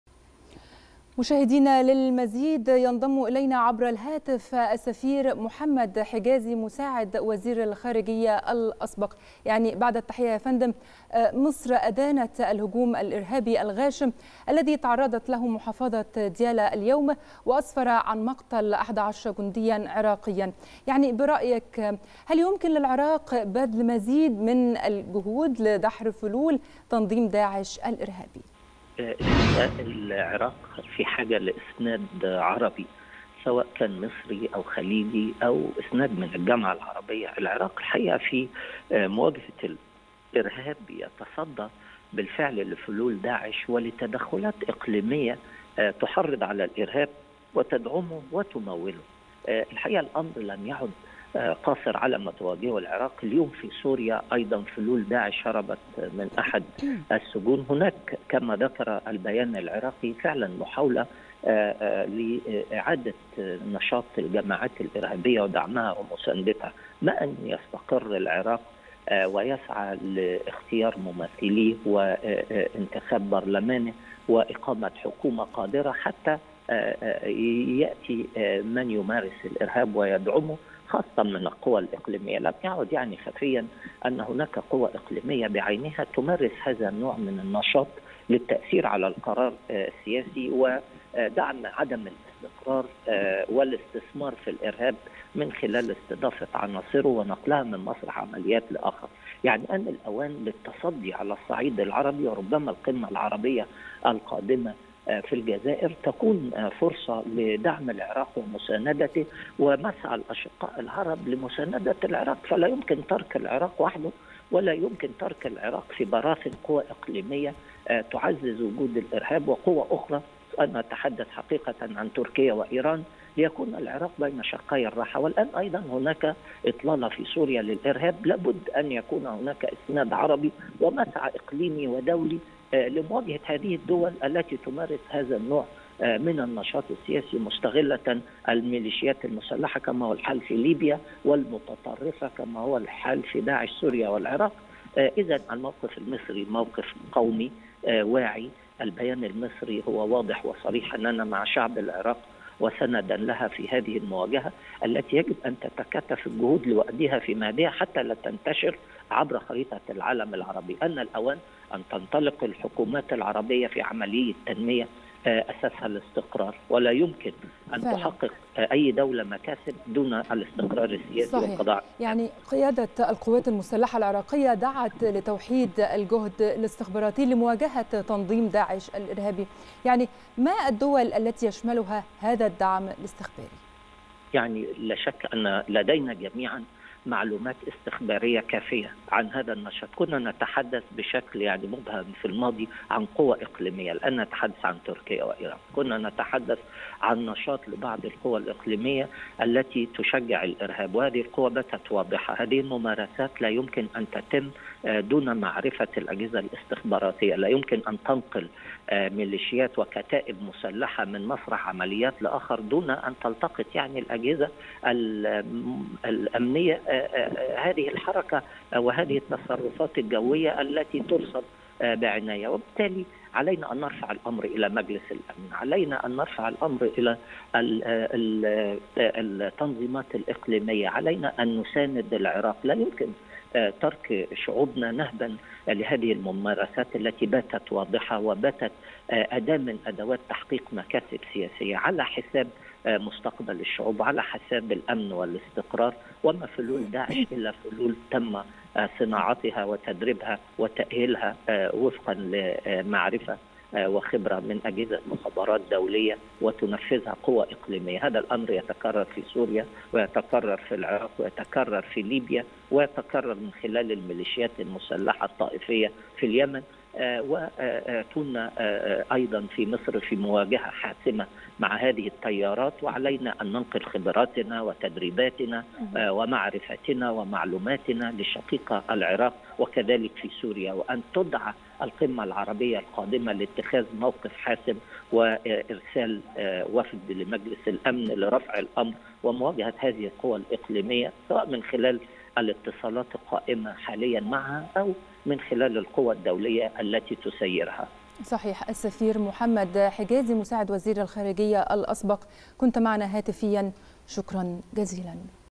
السفير محمد حجازي: العراق في احتياج إلى مساعدات عربية سواء من مصر أو الخليج للقضاء على الإرهاب
نشرة الثالثة | السفير محمد حجازي: العراق في احتياج إلى مساعدات عربية سواء من مصر أو الخليج للقضاء ء على الإرهاب